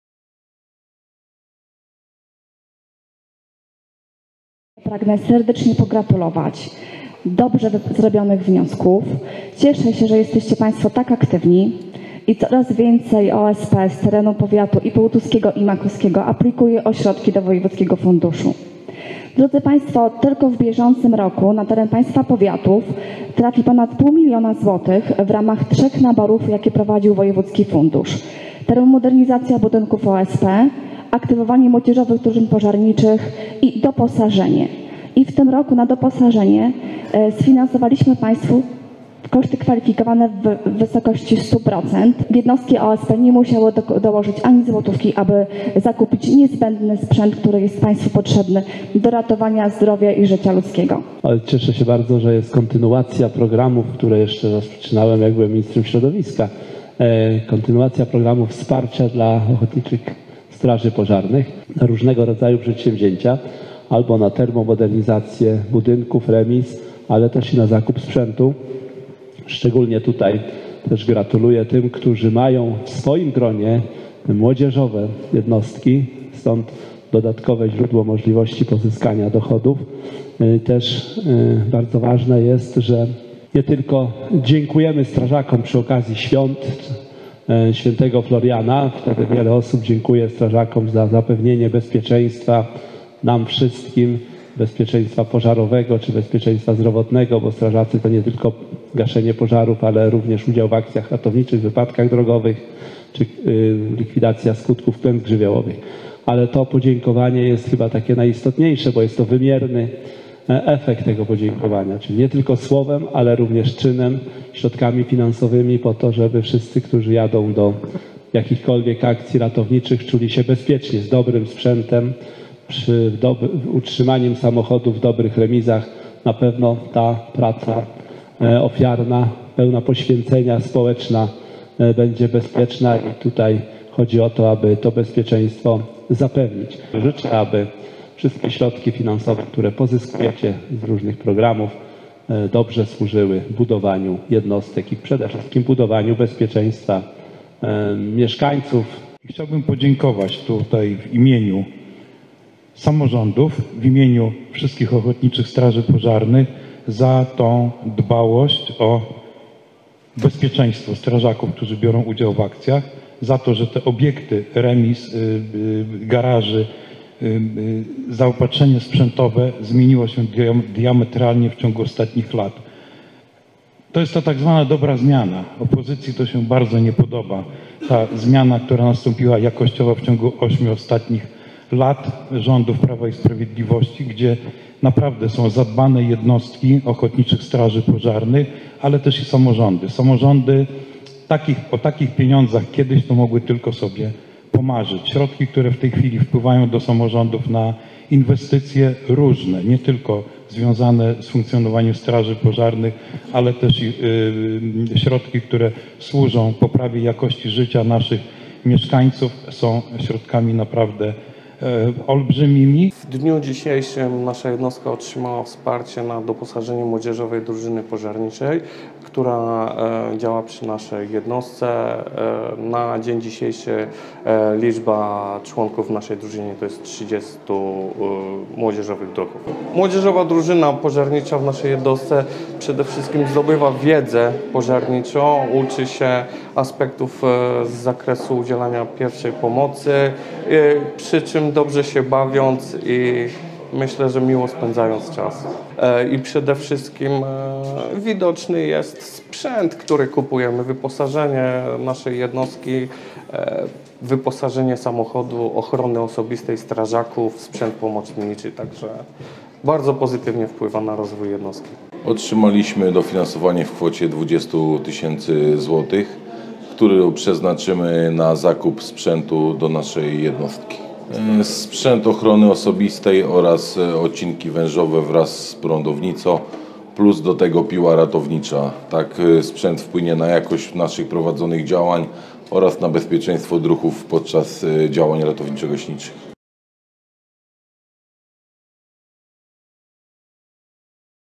Blisko pół mln zł z WFOŚiGW w Warszawie dla jednostek OSP z powiatów makowskiego i pułtuskiego
Jednostki otrzymają dotację na zakup nowego sprzętu i wyposażenia,  wsparcie młodzieżowych drużyn pożarniczych oraz termomodernizację budynków OSP i instalację paneli fotowoltaicznych. Dwadzieścia cztery umowy dotacyjne z beneficjentami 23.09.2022 r. w Pułtusku, podpisała Kamila Mokrzycka wiceprezes zarządu Wojewódzkiego Funduszu Ochrony Środowiska i Gospodarki Wodnej w Warszawie. W wydarzeniu wziął udział wicepremier, minister rolnictwa i rozwoju wsi Henryk Kowalczyk, a także lokalni samorządowcy i parlamentarzyści.